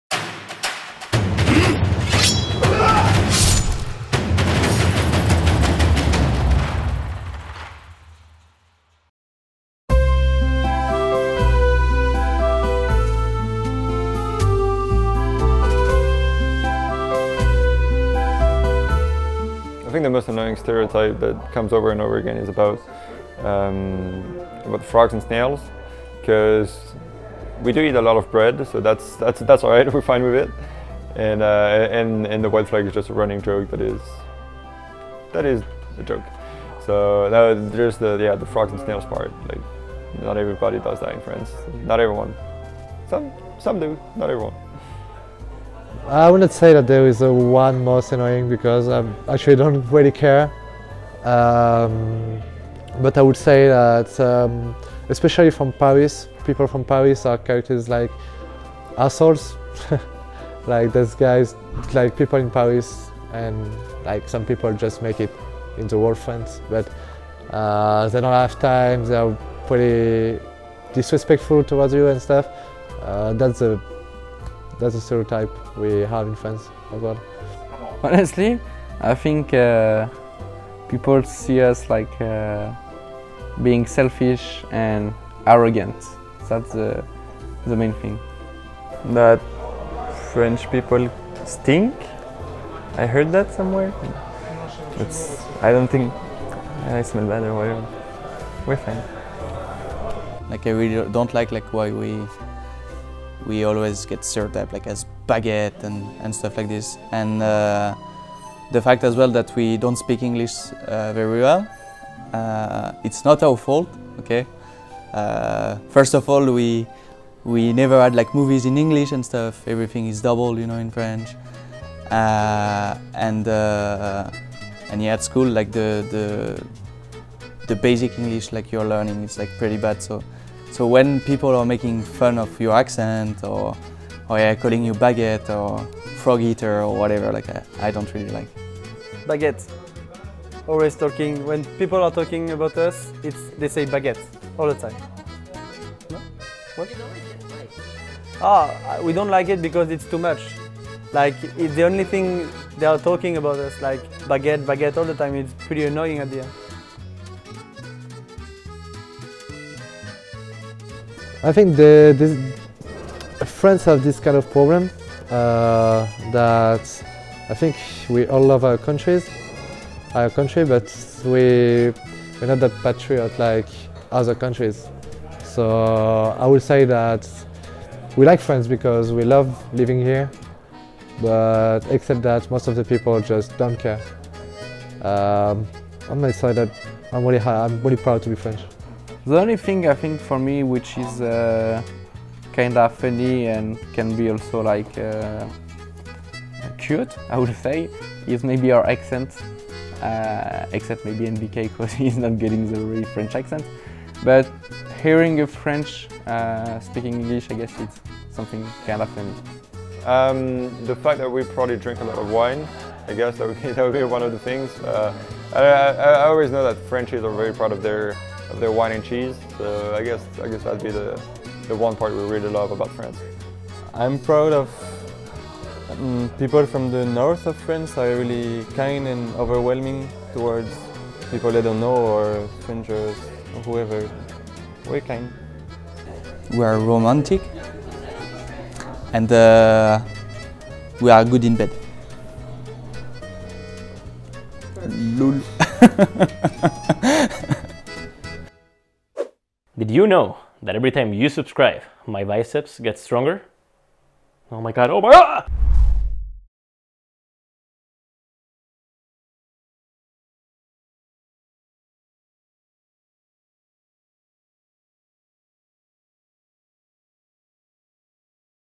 G2 Esports: Vive La France! shox, kennyS and others offer their take on the French stereotypes
France - romantic, famous for its melodic language, wine, cheese and CS:GO players... During the CS:GO mid-season break we decided to bring you a video where G2 Esports CS:GO players and their coach SmithZz discuss the stereotypes about French people which bother them and the things they are proud of.